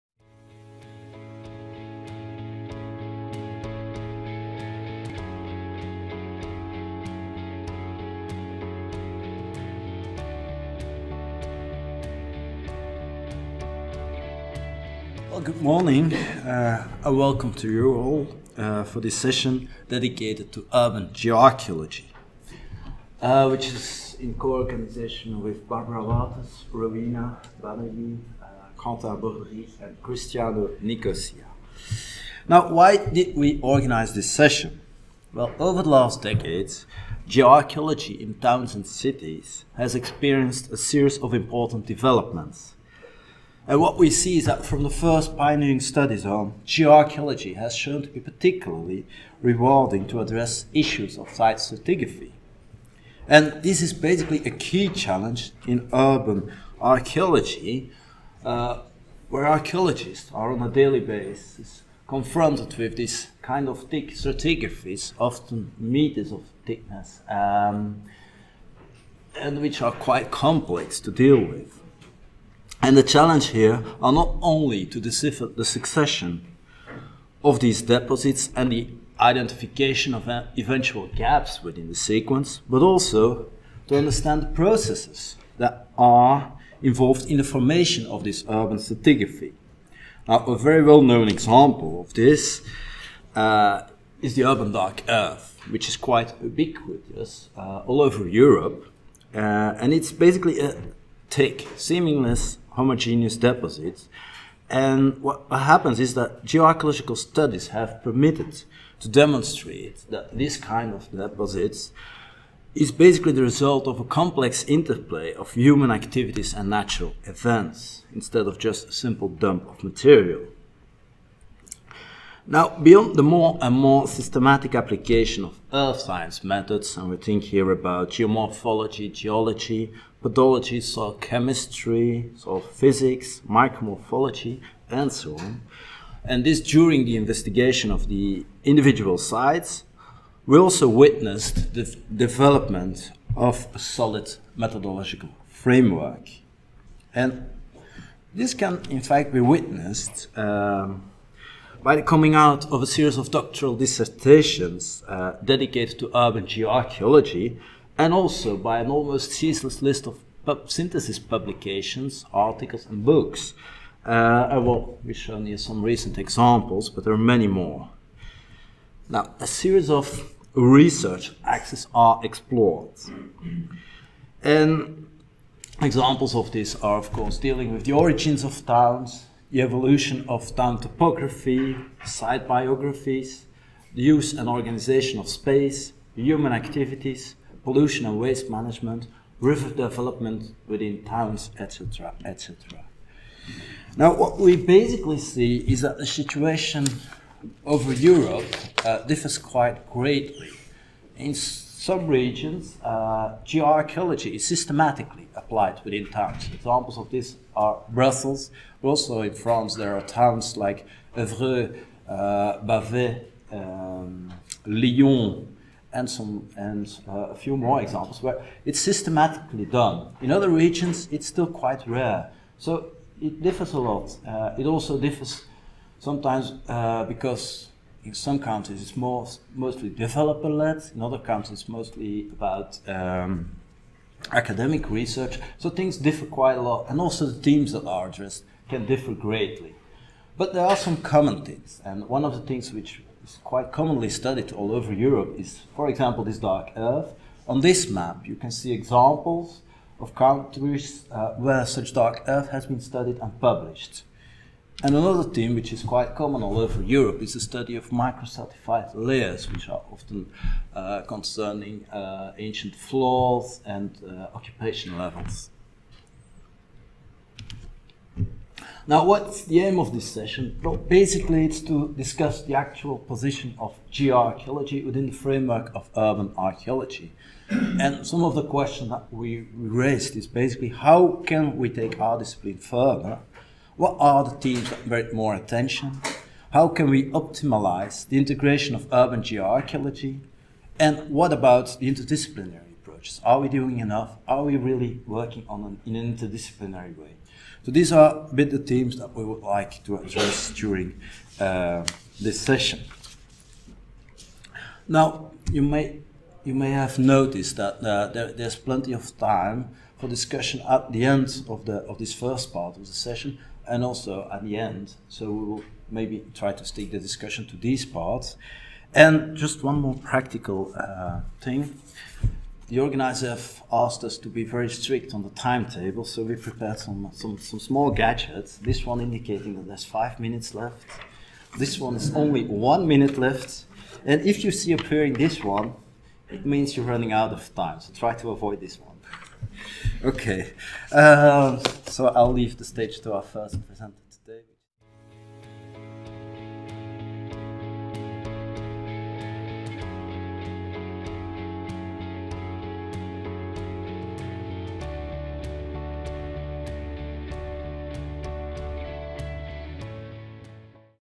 536 URBAN GEOARCHAEOLOGY: INTRODUCTION
Geoarchaeological research, the application of earth science methods and concepts (geomorphology, geology, pedology, soil chemistry, mineralogy, etc.) in urban contexts has experienced an important development over the last decades. Especially the UK, France and Italy have longstanding traditions, but urban geoarchaeology has more recently also successfully been introduced in for example Belgium and the Baltic states, for example. In this introductory talk we intend to give a brief state of the art of urban geoarchaeology in different parts of Europe. We will touch upon the many different methods that have been employed, the need for multiscalar approaches ranging from the geomorphological study of the town and its surroundings to the micromorphological study of individual stratigraphic units, and how geoarchaeological research is integrated in the broader archaeological framework. We will further address a series of current topics, such as the impact of urbanization on soils; the use and organisation of urban space; urban stratigraphy and Dark Earth; ancient soil pollution in towns; reconstructions of the physical (geomorphological) environment, integrated environmental research involving close interaction with archaeobotanists and archaeozoologist, for instance, and the interactions of towns within their wider regions. Finally, we will discuss some of the challenges faced by geoarchaeological research in urban contexts.

Author(s): Devos, Yannick (Université Libre de Bruxelles) - Wouters, Barbora (Vrije Universiteit Brussel; Aarhus University) - Banerjea, Rowena (University of Reading) - Borderie, Quentin (Département de l’Eure- et-Loir) - Nicosia, Cristiano (University of Padova)